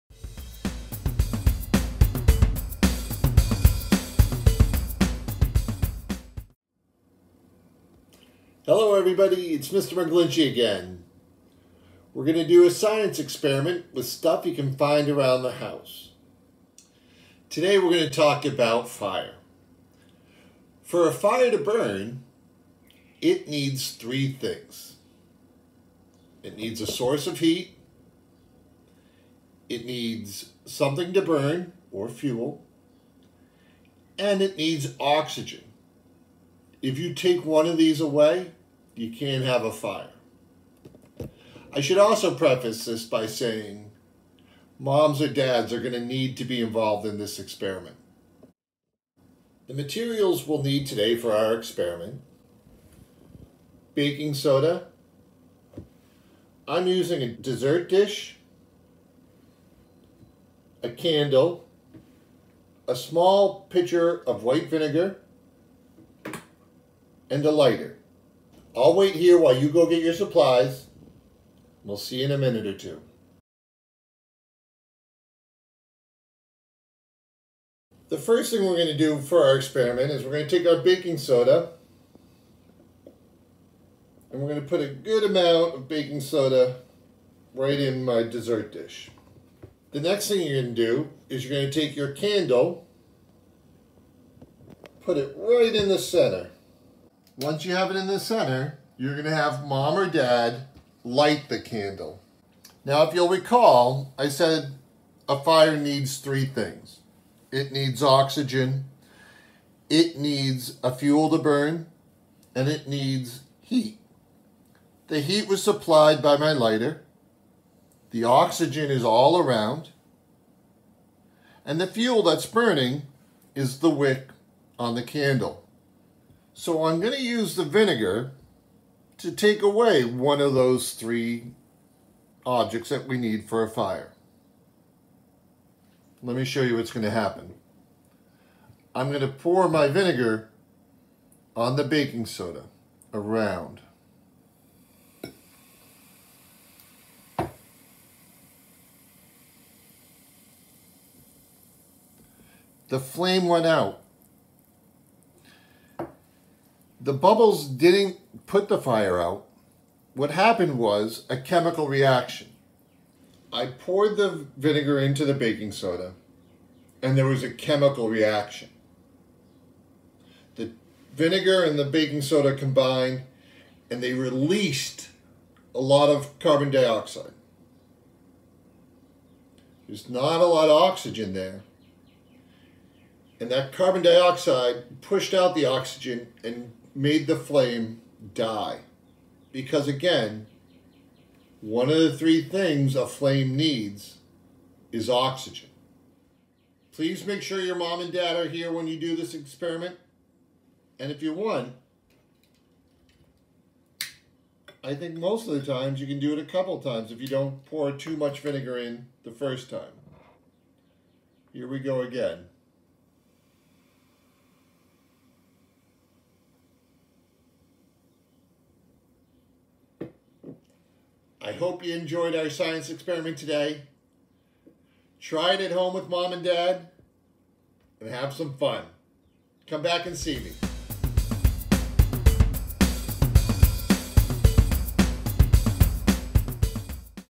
Extinguishing Fire
In this experiment a chemical reaction will be used to put out a candle flame.